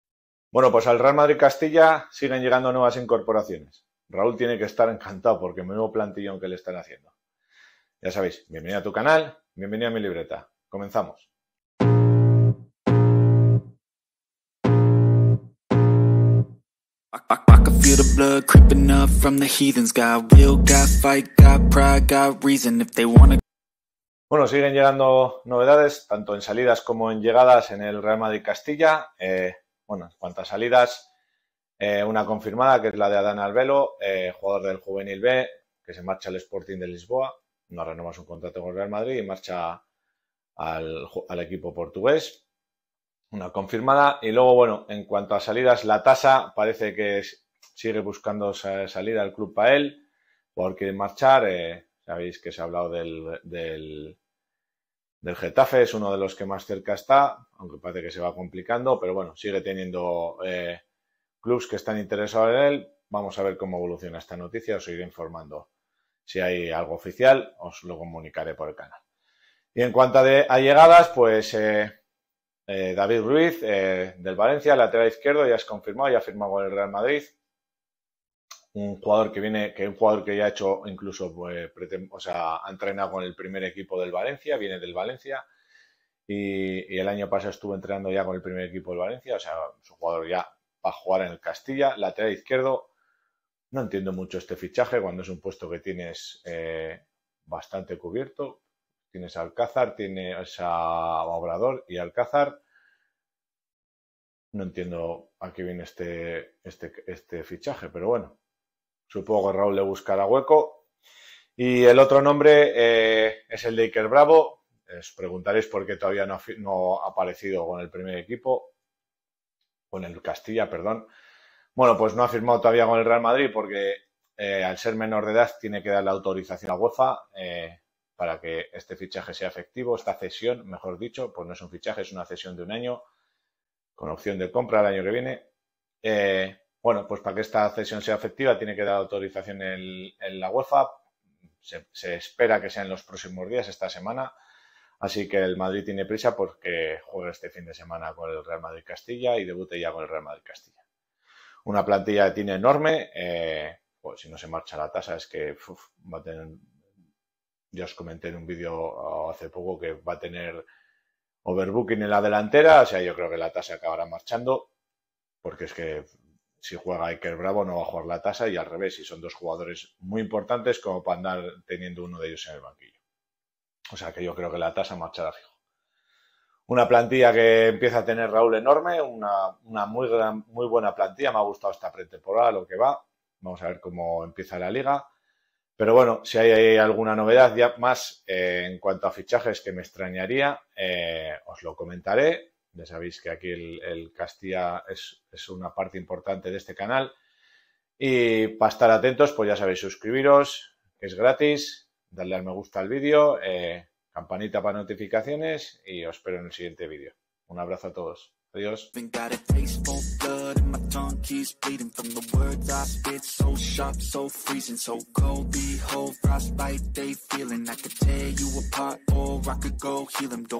🍼🎙 El fichaje. de IKER BRAVO en manos de la UEFA.
Salidas y llegadas en el equipo de Raúl. El RM. Castilla perfila su plantilla y espera que se pueda confirmar el fichaje del gran delantero del Bayer Leverkusen en las próximas horas.